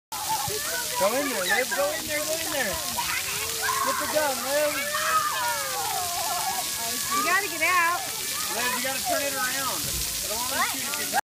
Splash Pad at Creekside Park in Alpine Utah
Best place in Alpine to enjoy a lazy summer day and get wet in the splash pad.